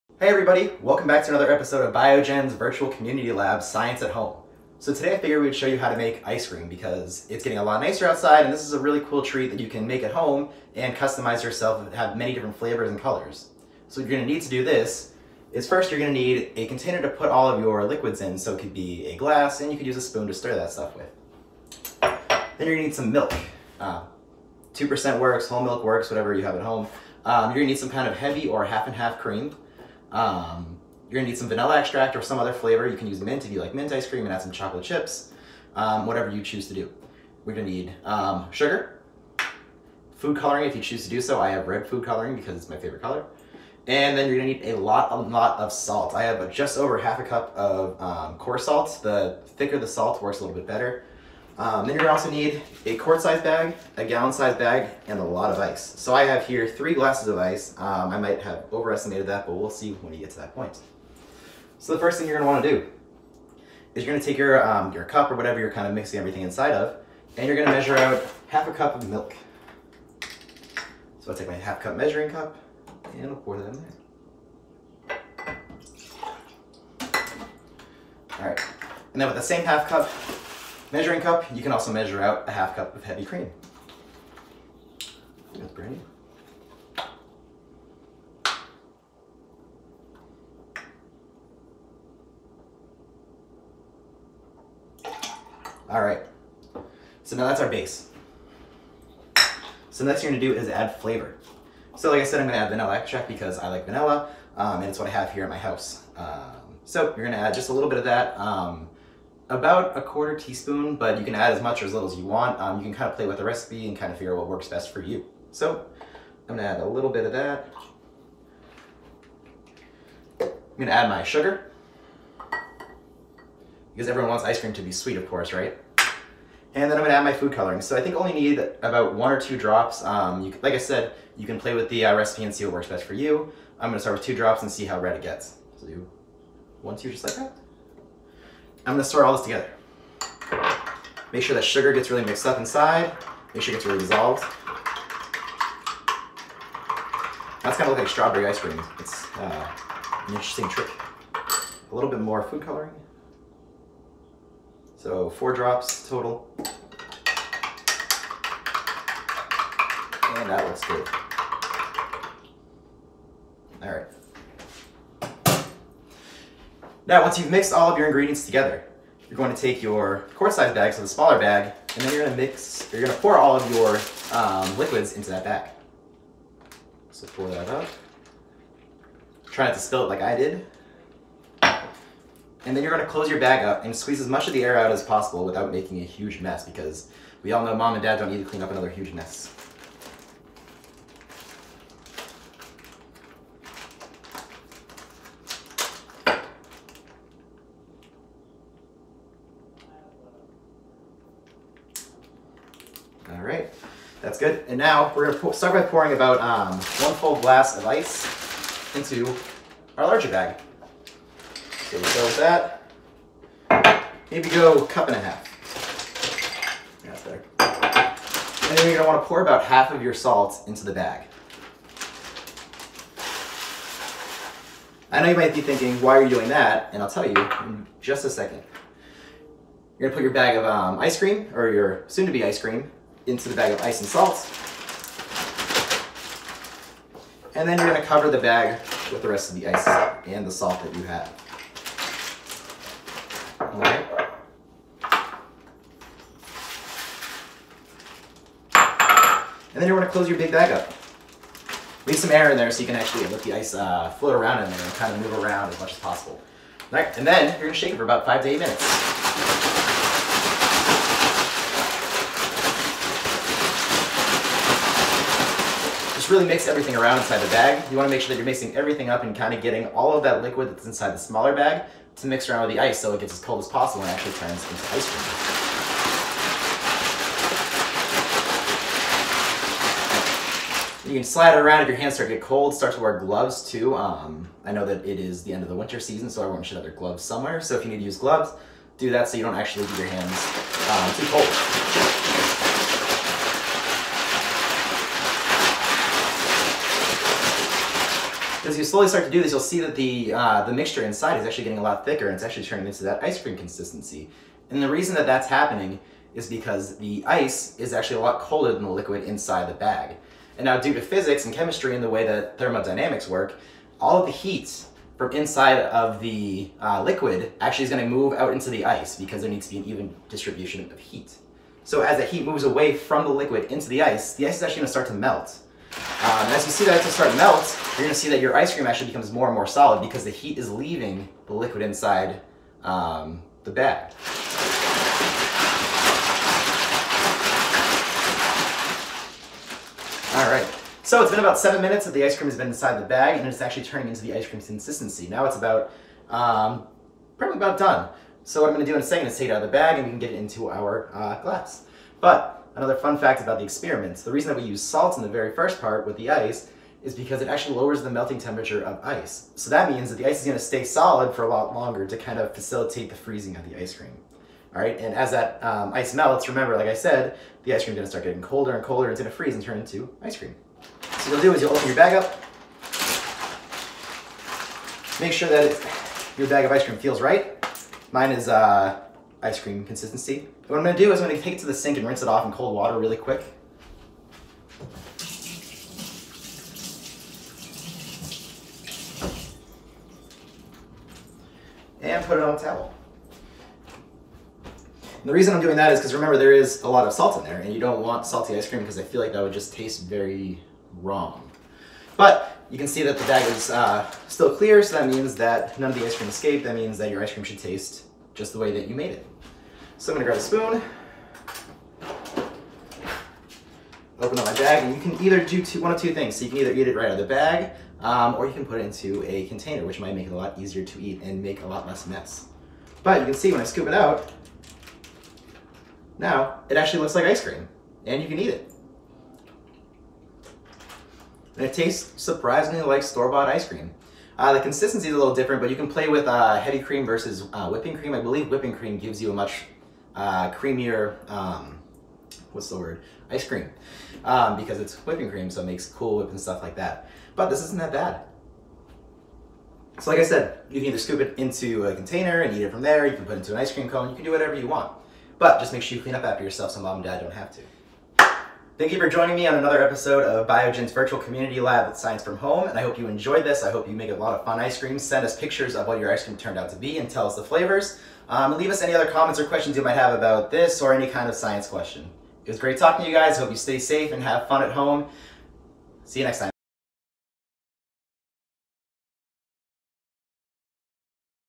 Virtual Community Lab: DIY Ice Cream
Learn how to DIY ice cream in your kitchen by following along with Connor Haney, a Biogen Community Lab member.

Twitter.com/Biogen
Facebook.com/Biogen